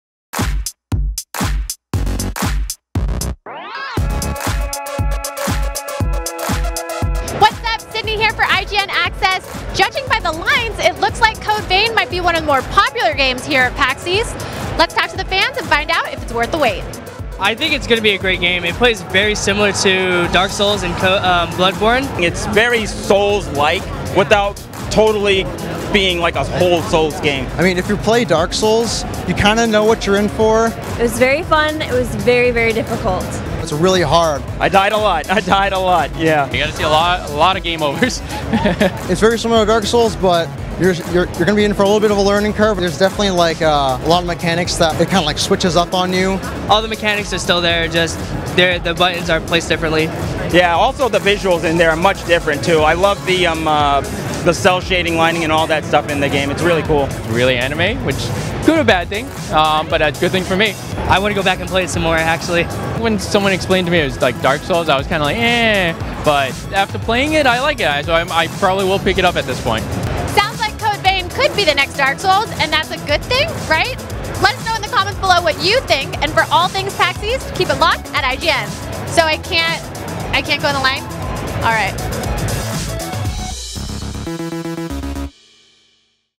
Is Code Vein the Next Dark Souls? Here's What the Fans Thought - IGN Access
Code Vein had a huge turnout at Pax East 2018 but do fans think it's the next Dark Souls contender?

Catch up on Everything PAX 2018!

Follow IGN for more!